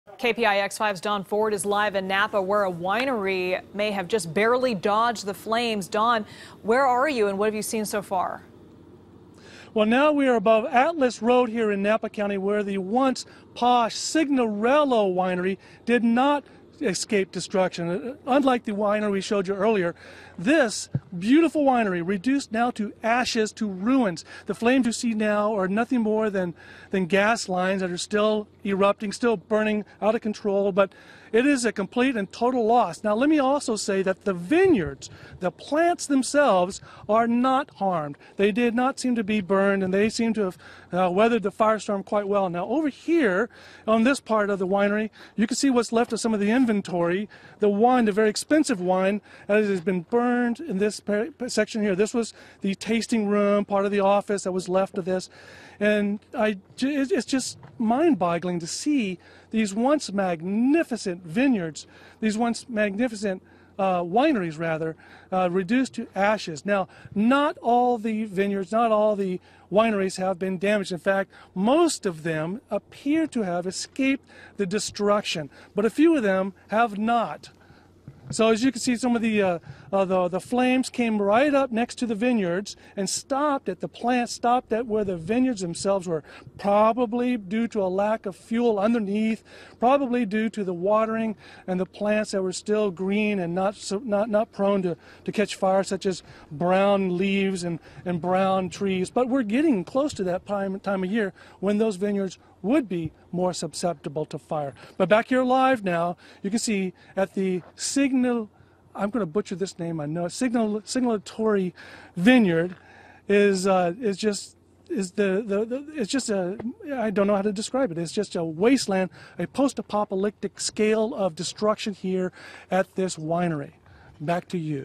Signorello Estate Among Wineries Destroyed In Napa
Don Ford reports from ashes of destroyed Signorello Estate winery (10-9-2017)